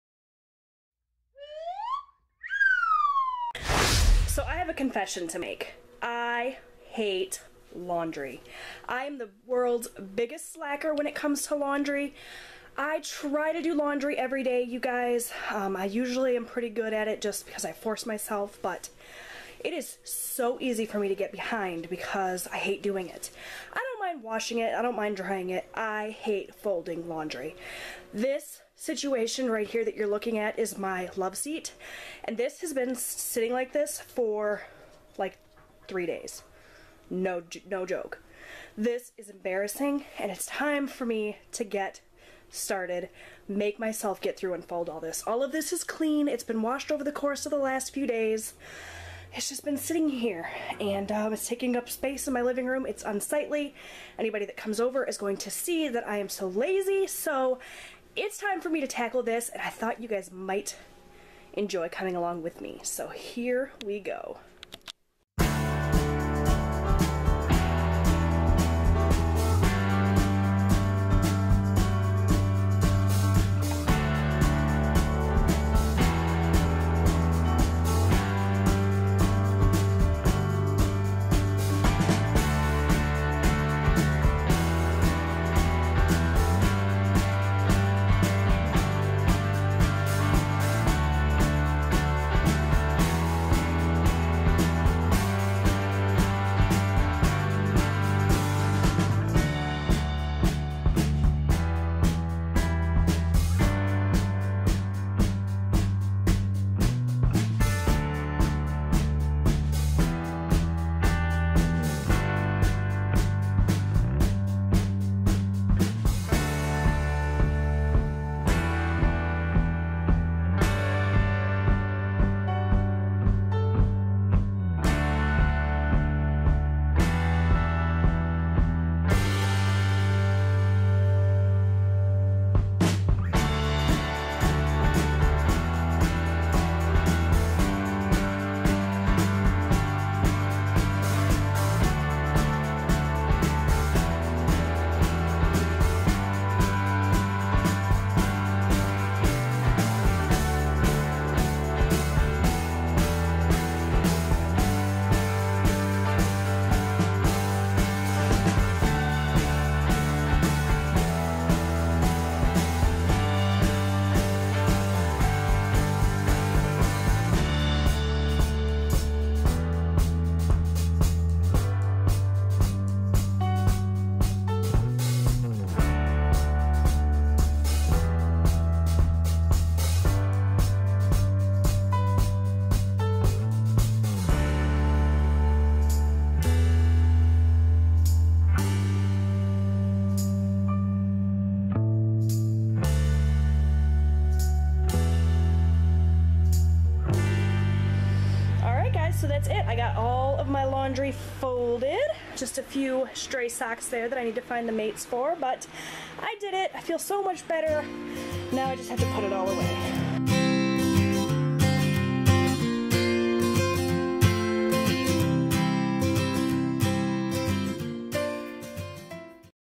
Tackling the Laundry Pile (Speed Cleaning)
I have a confession to make. I am not the neatest person in the world. I am also not the most easily motivated. It's not unlike me to leave things undone. Especially the laundry! I don't mind washing and drying the laundry. But I hate folding it! So in this video I tackle the pile of clean clothes that has been staring me in the face for an embarrassing 3 days. I thought you might get some motivation by watching this. Enjoy!

If you have any other video requests, let me know down below as well!!

Let's be penpals! If you send me a letter or card I'll send one back!
Jwhackers
Carleton, Mi
48117